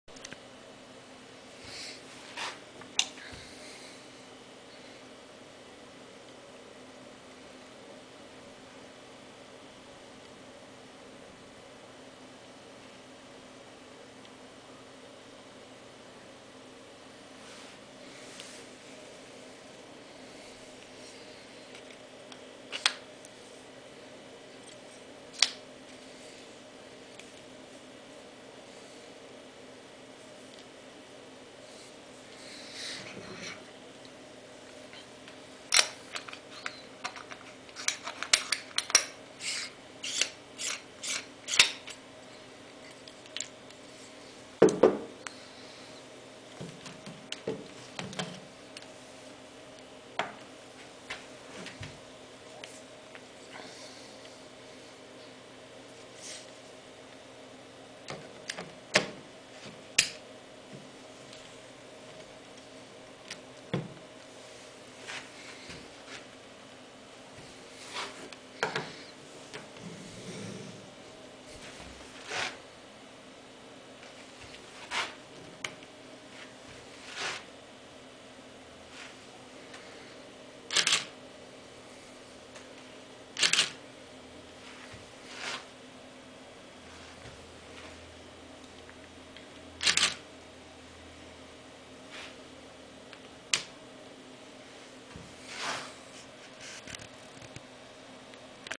Jupiter-21m 200mm russian lens + Nikon D80 cmaera
Jupiter-21 M - the telephoto lens is made on Kazan factory in Kazan (Russia). This lens with a jumping diaphragm is intended as a replaceable lens for cameras of type "Zenit" and "the Practika", having the device for automatic closing of a diaphragm at shooting.

A lens can be photographed from a long distance, to use it for photohunting and shootings of architectural details. Shooting can be made on black-and-white and a coloured film.

The Jupiter-21 m lens has the press automatic diaphragm steered the gear of the camera. It can be applied and other mirror cameras with a shot format 24х36 mm, connecting thread М42х1, a working piece 45, 50 mm, having and not having the device of automatic control of a press diaphragm. High resolving force of a lens allows to receive in a picture close up fine details of remote subjects. Rather big light power gives the chance shootings of sports plots from a hand at endurance 1/250 - 1/500 seconds.